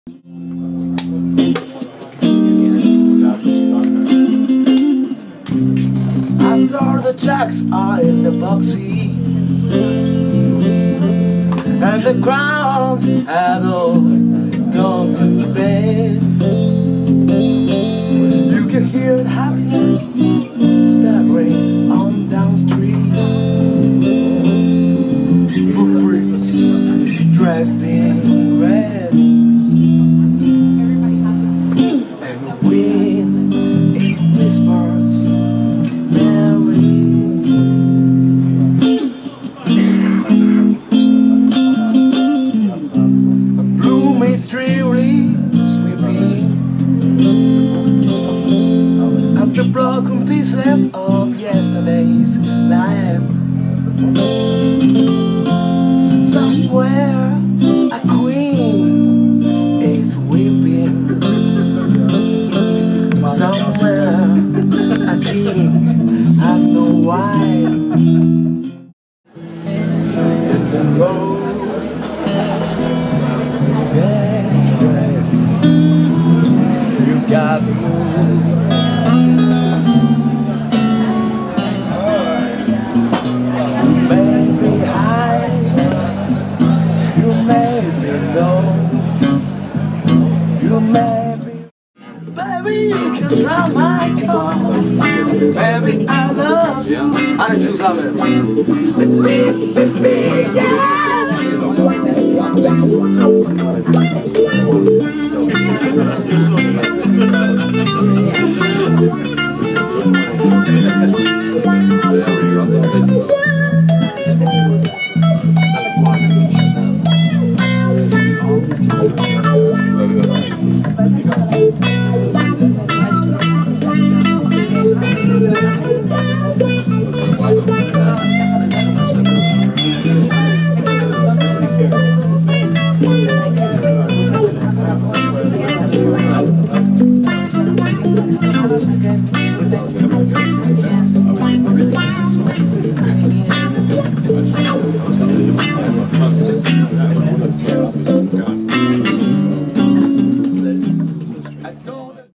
Kota "Hendrix" Yoshioka
12. august 2016, KOTA in concert gesang und gitarre, mit yessie am bass...